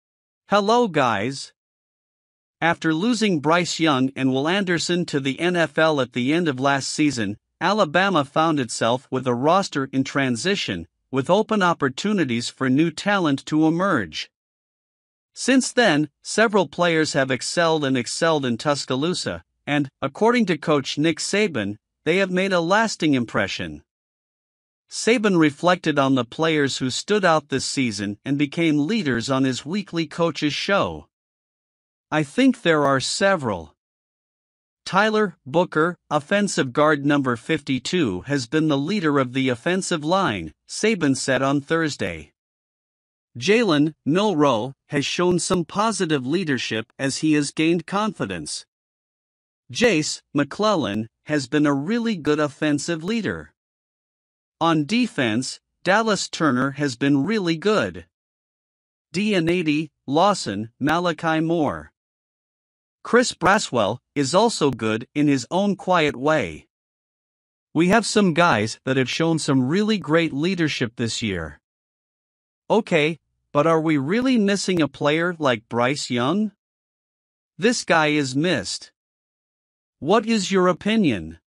DID ALABAMA LOSE ALABAMA FOOTBALL UPDATE!
Hello guys!

After losing Bryce Young and Will Anderson to the NFL at the end of last season, Alabama found itself with a roster in transition, with open opportunities for new talent to emerge. Since then, several players have excelled and excelled in Tuscaloosa, and, according to coach Nick Saban, they have made a lasting impression.